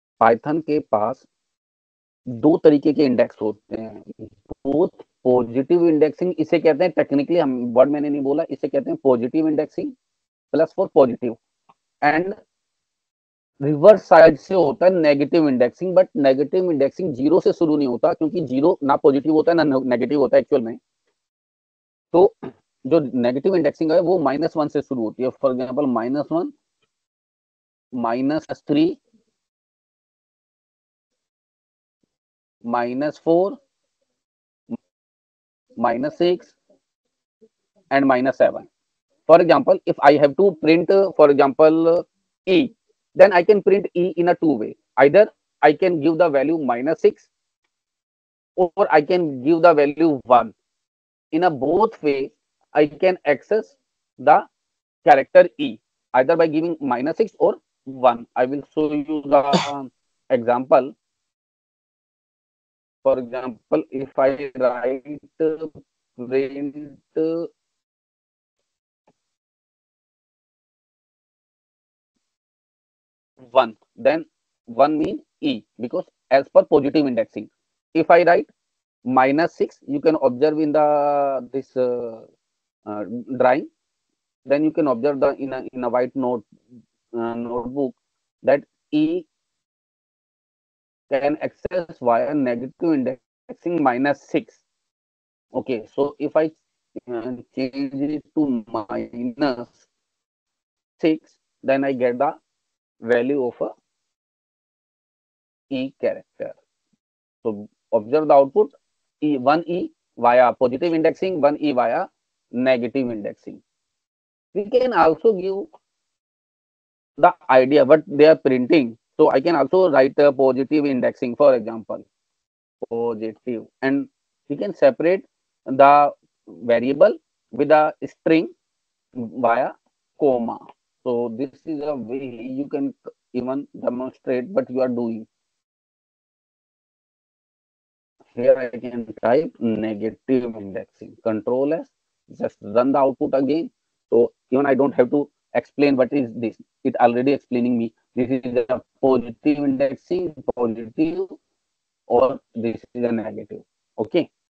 10.2 Types of Indexing in string | Python programming language
In this video, we explore the different types of indexing in Python strings. Python supports two types of indexing:

1. positive indexing – starts from 0 and moves forward.
2. negative indexing – starts from -1 and moves backward from the end of the string.

We demonstrate how both types work with practical examples and show how to use them effectively in Python programming.